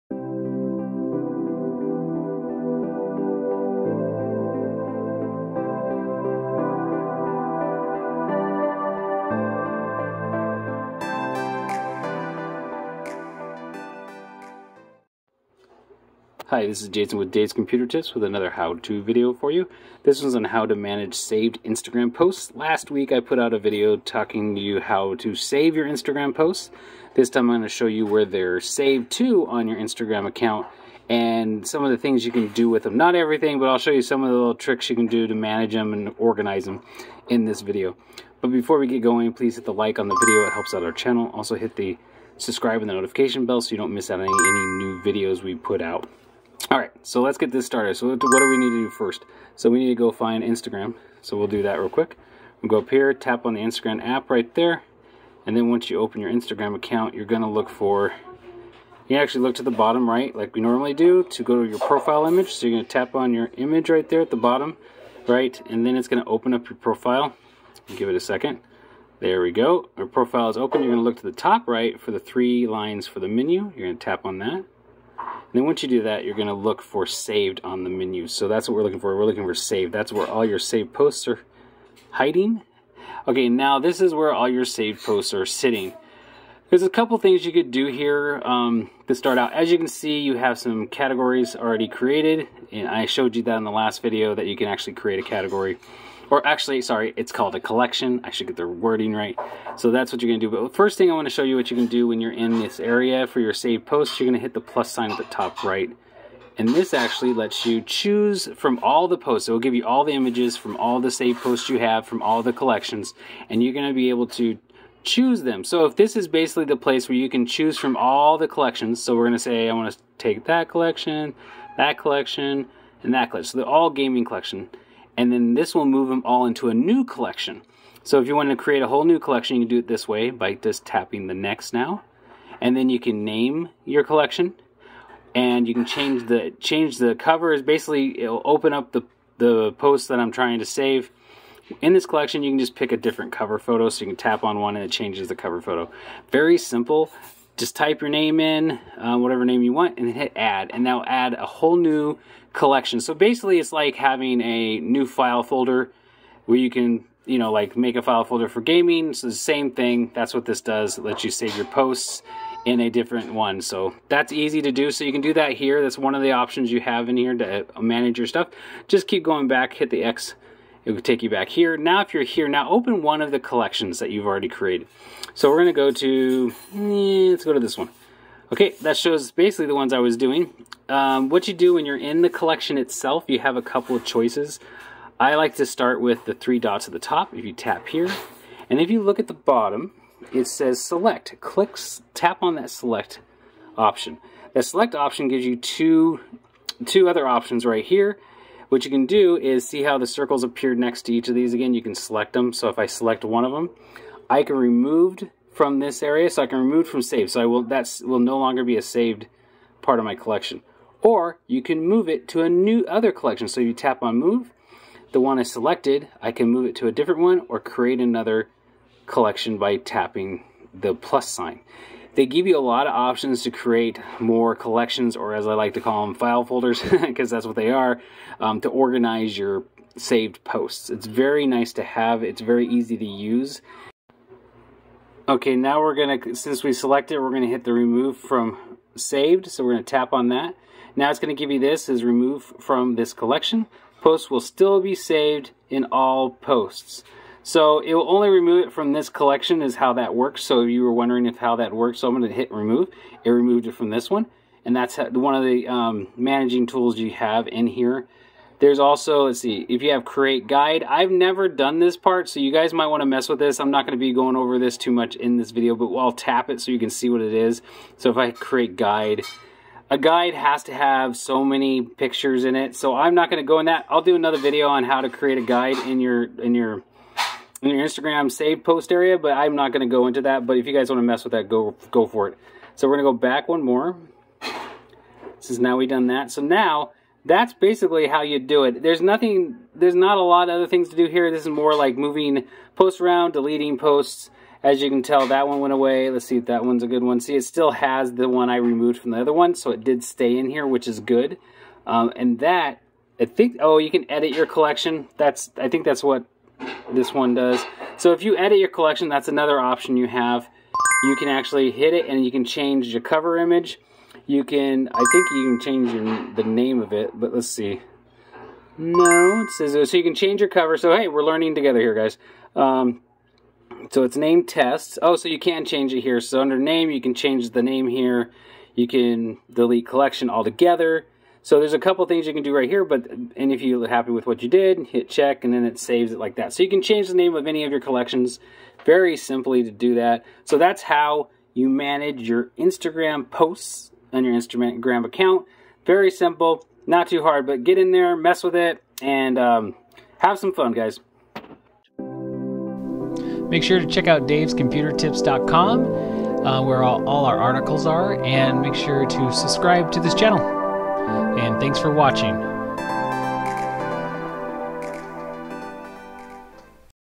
How To Manage Saved Instagram Posts
In this video I will be showing you how to Manage those Saved Instagram posts.
Many of us use the app, Instagram, for posting food pics to selfies of our daily lives. Last week’s video I told everyone how to save those posts you LIKED so they be could found later. If you missed that one, here is the link to it:

Now that we are caught up on that, this week I wanted to show my readers how you can manage those Saved posts. We showed at the end of the last article where you can find them after they were saved. Now it is time to show everyone some of the management features of this area. Time to pull out those devices and learn something new.